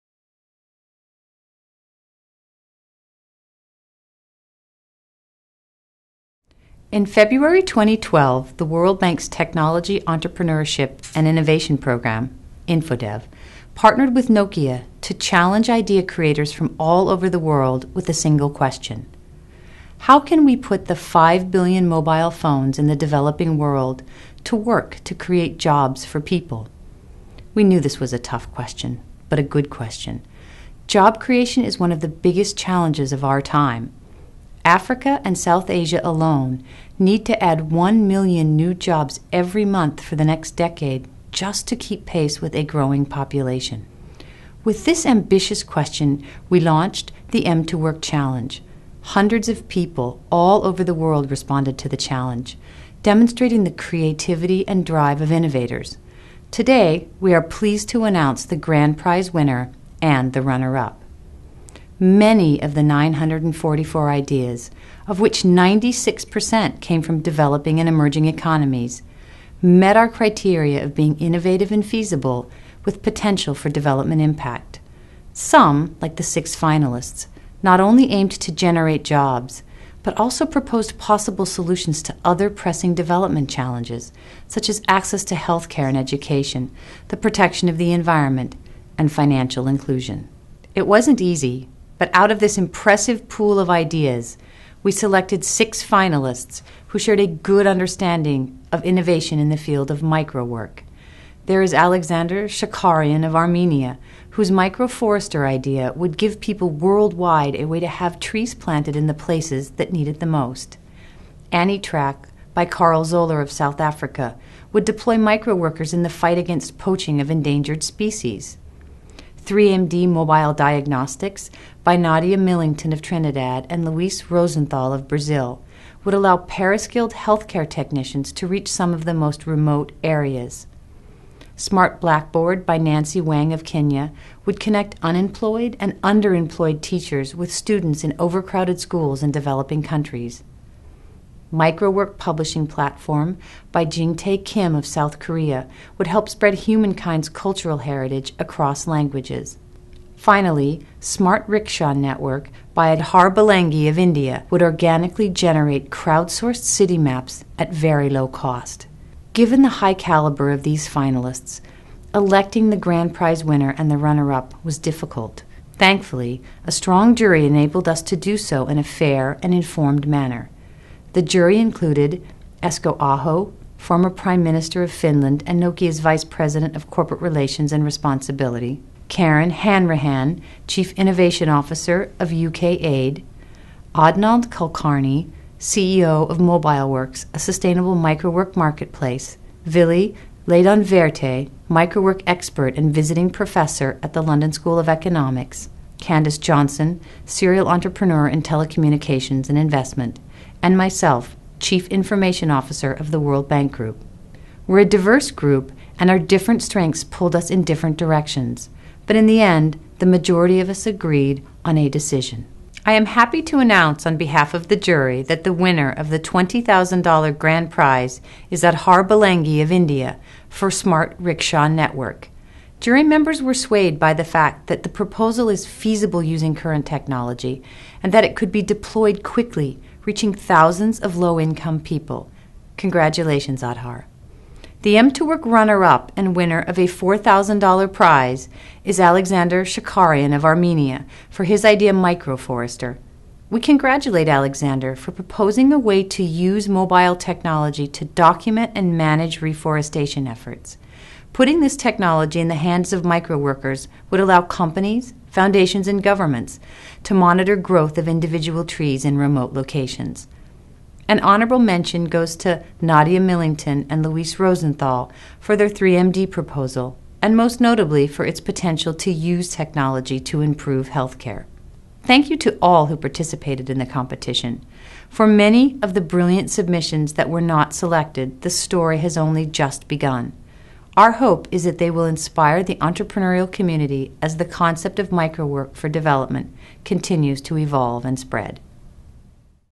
m2Work - Grand Prize Winner announcement
Stephanie von Friedeburg, Chief Information Officer of the World Bank Group and chair of the m2Work jury, announces Aadhar Bhalinge as the winner of m2Work challenge's $20,000 grand prize for his idea "Smart Rickshaw Network." The app would crowdsource maps at a very low cost in developing nations by employing fleets of rickshaw drivers to feed live traffic updates into a subscription service.

Second place went to Armenia's Alexander Shakaryan, whose "MicroForester" app would aid reforestation projects. Nadia Millington and Luis Rosenthal got an honorable mention for "3MD: Mobile Diagnostics" which would allow paraskilled technicians to perform disease diagnosis tasks on patients' digitized scans.

The competition organized by Nokia and infoDev drew a total of 939 ideas [944 in total, minus 5 double submissions], 96% of which came from developing and emerging economies. m2Work, which stands for mobile microwork, aims to expand microwork to the 5 billion mobile phones in the developing world. Currently, millions of people supplement their income through microwork—small digital tasks they can perform online.